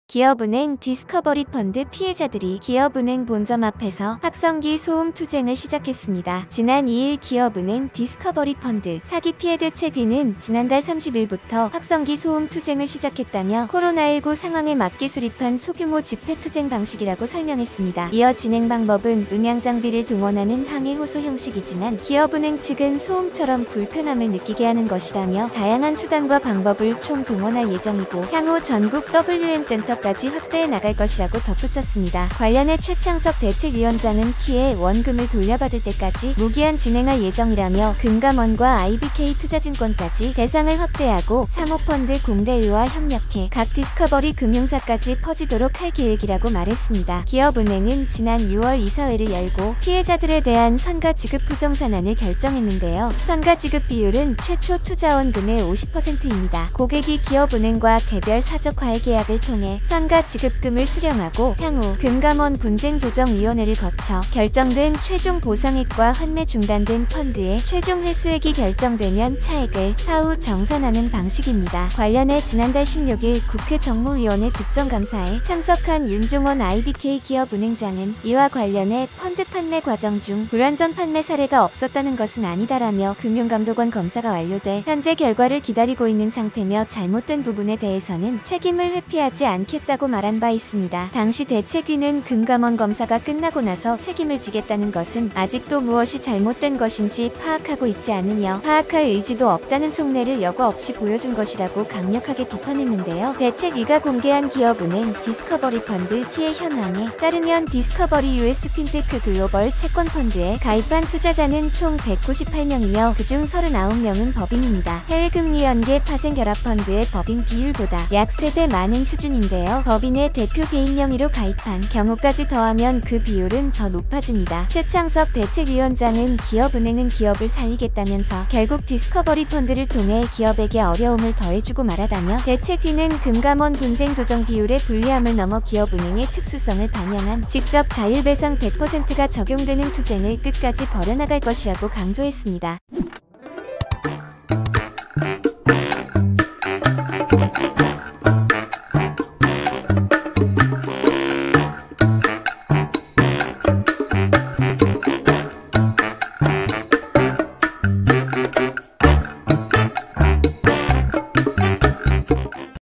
확성기 들고 소음 투쟁에 나선 기업은행 디스커버리펀드 피해자들
기업은행 디스커버리펀드 피해자들이 기업은행 본점 앞에서 ‘확성기 소음 투쟁’을 시작했다.